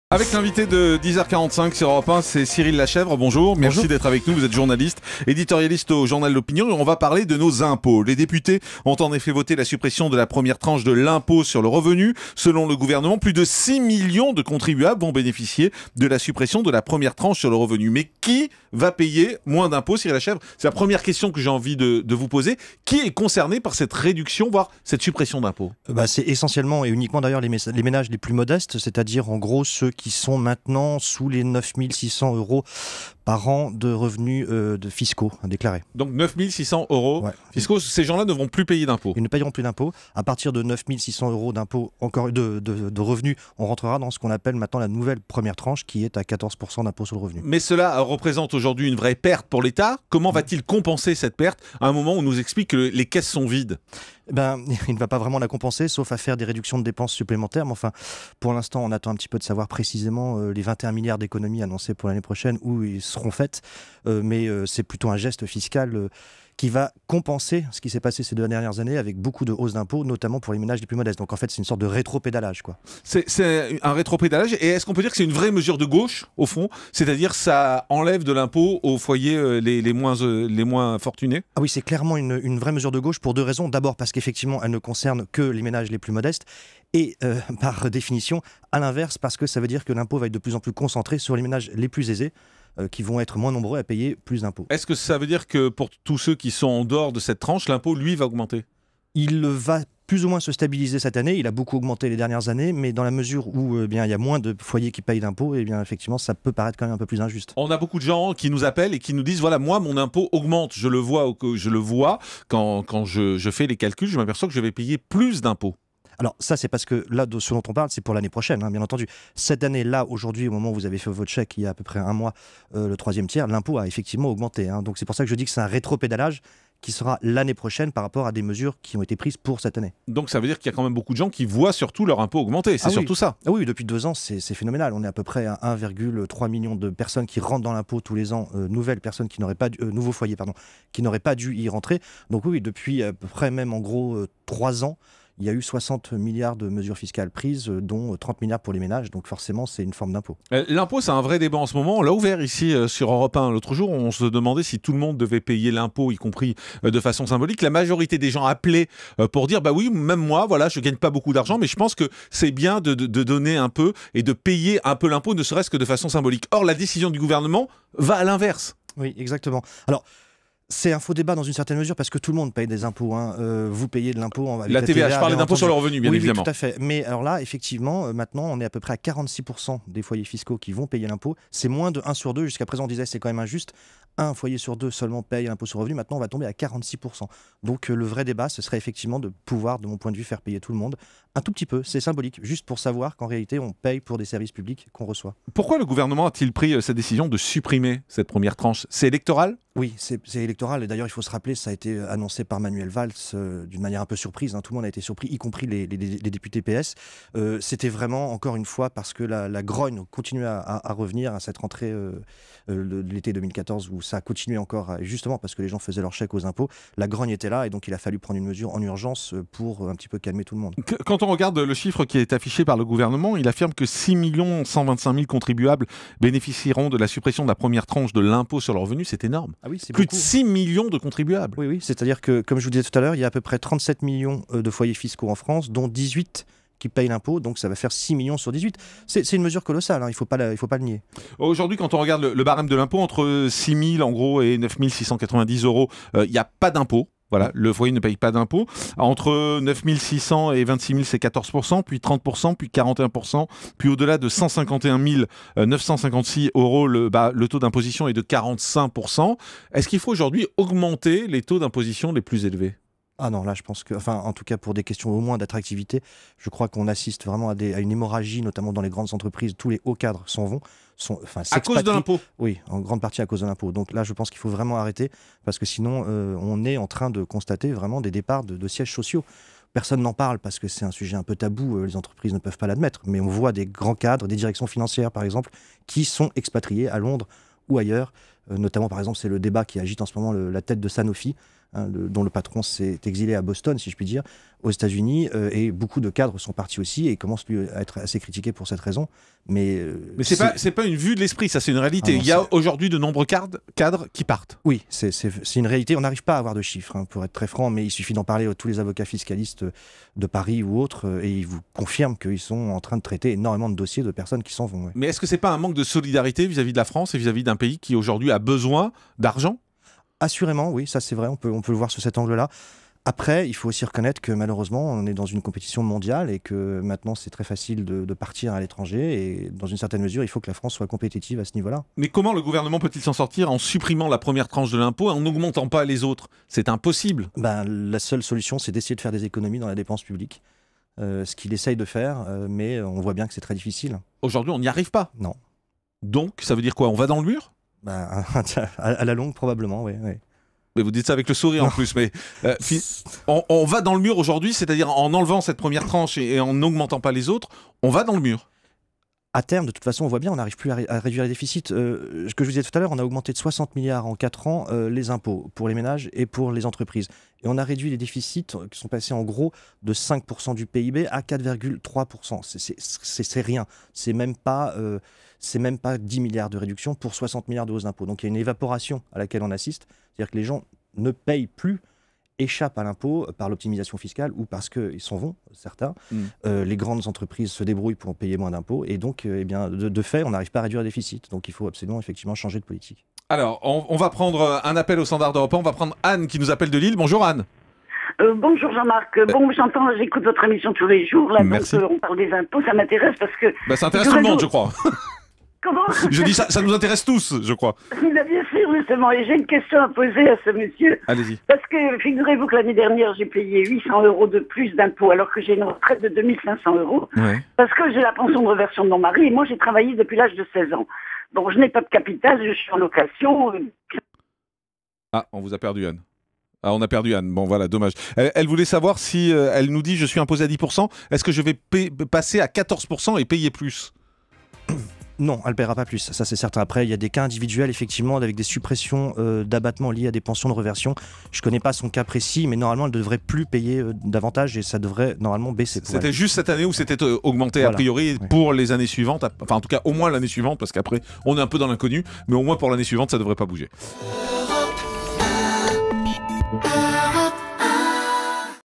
Les gagnants et les perdants du recalcul de l’impôt
Les députés ont voté la suppression de la 1e tranche de l'impôt sur le revenu. Cyril Lachevre, journaliste et éditorialiste à L'Opinion, nous explique quels ménages vont en bénéficier.